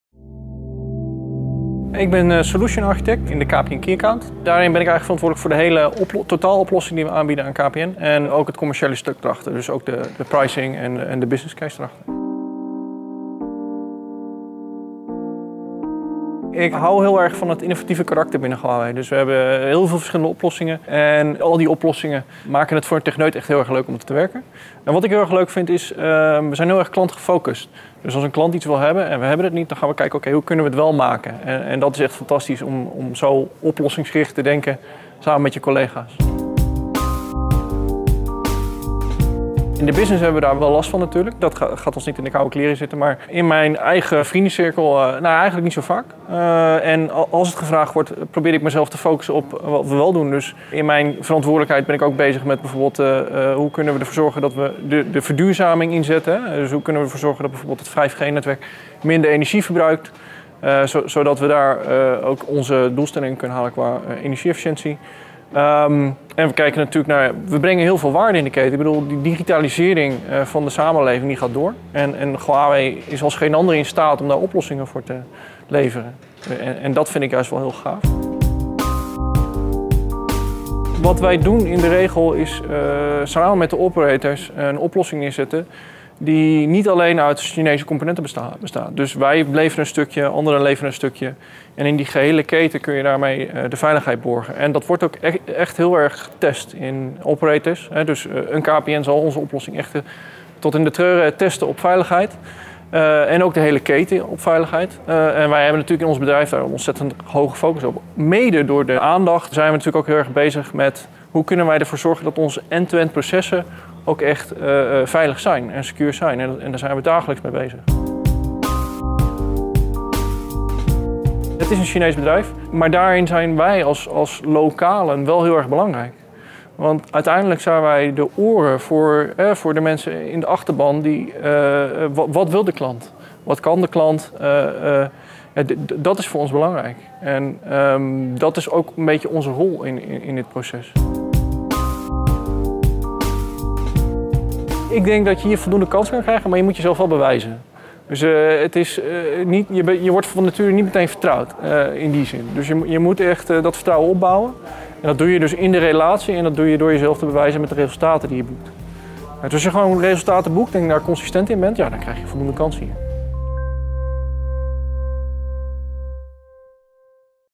1. Stephan de Jonge | Maak kennis met onze medewerkers
Stephan vertelt over zijn ervaringen bij Huawei: “Ik houd heel erg van het innovatieve karakter van Huawei. We hebben voor onze klanten veel oplossingen en waar die er niet zijn, verzinnen we ze. Dat maakt het voor een techneut heel erg leuk om hier te werken.”